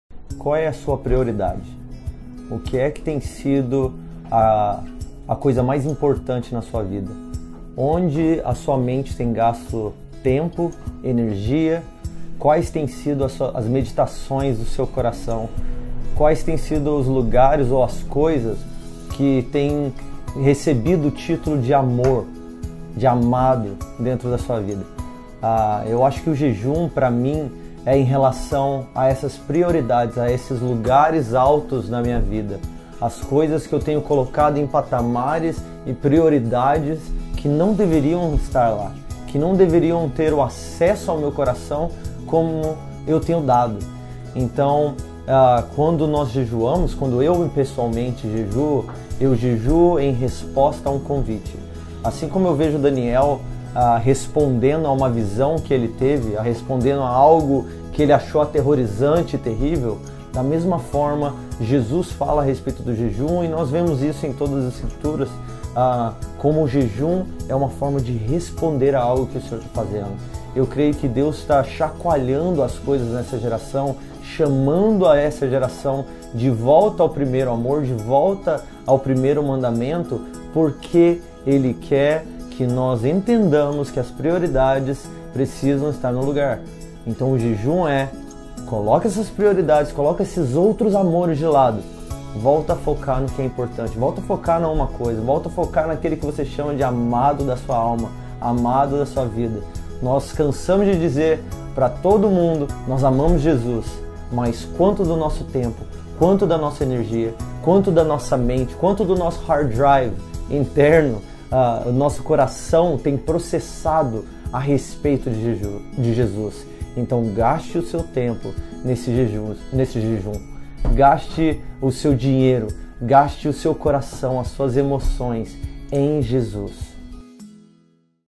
Romper 2017 | Estamos Juntos!
Mesmo em dias difíceis, temos grande convicção de que existe solução para os problemas em nossa nação. Por isso, durante 21 dias estaremos juntos, como comunidade orando e jejuando por um romper pessoal e também pelo Brasil.

O Brasil precisa de uma manifestação da Glória de Deus e de uma restauração em todas as suas esferas. Mas, diante de Deus e juntos, clamaremos para que nós sejamos resposta neste tempo. Para sermos aqueles que irão contemplar Jesus e resplandecer quem Ele é à nação.

Junte-se a nós de 23 de Abril a 13 de Maio.

É chegada a hora de romper!